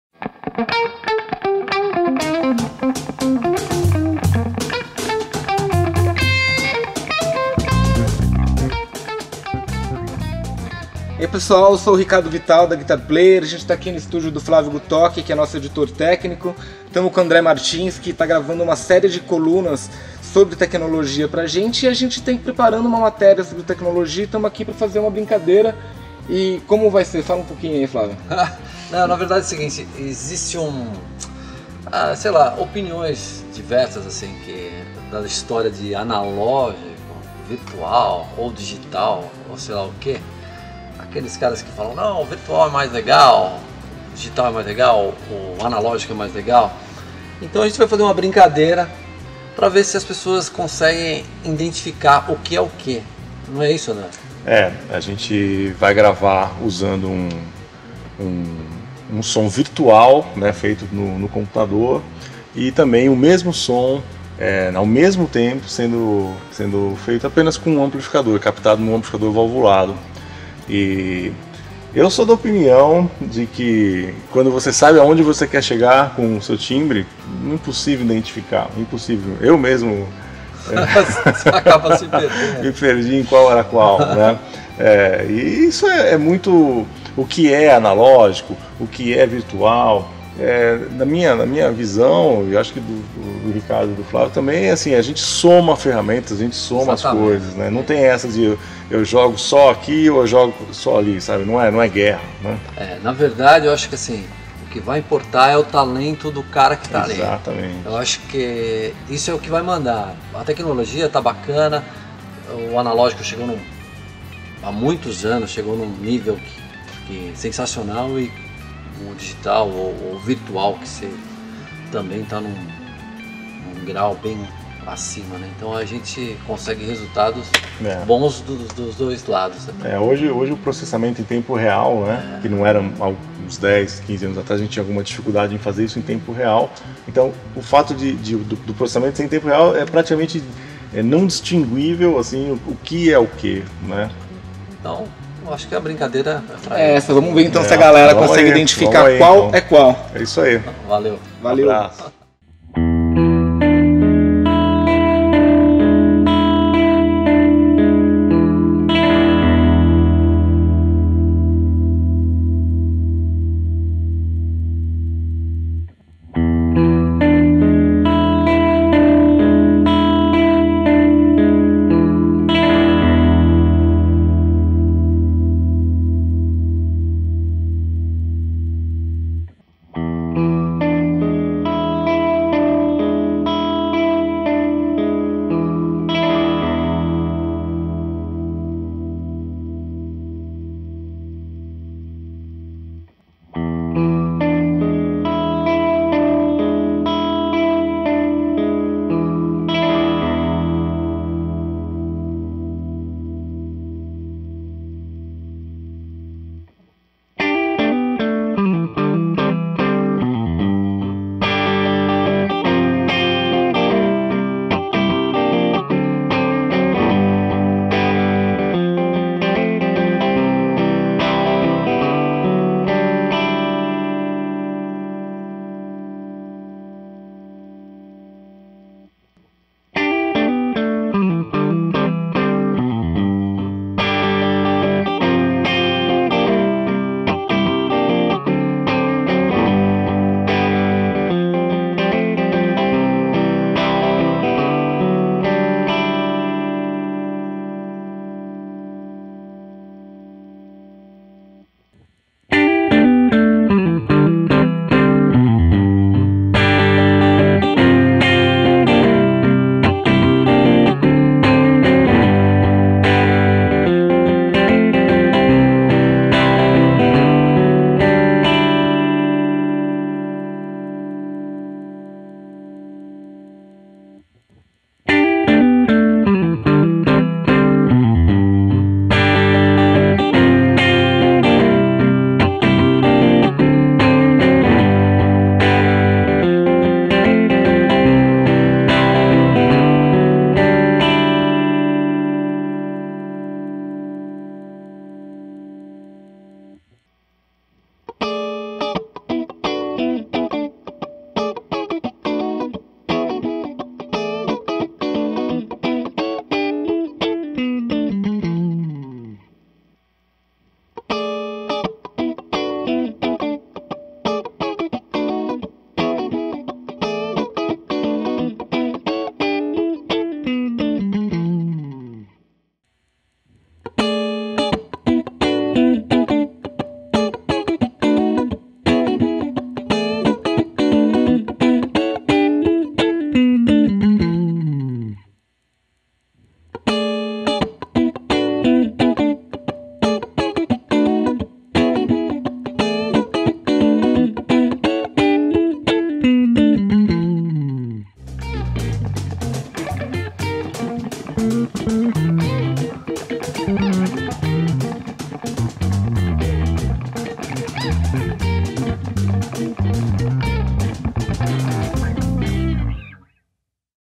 Guitar Player Brasil - Edição #213 - Analógico vs Virtual
Ricardo Vital, Flávio Gutok e André Martins comparam recursos analógicos e virtuais, compartilhando a experiência com os leitores da Guitar Player Brasil com o intuito de complementarem a matéria "High-Tech Ou Vintage" da edição #213 (Janeiro de 2014) da revista.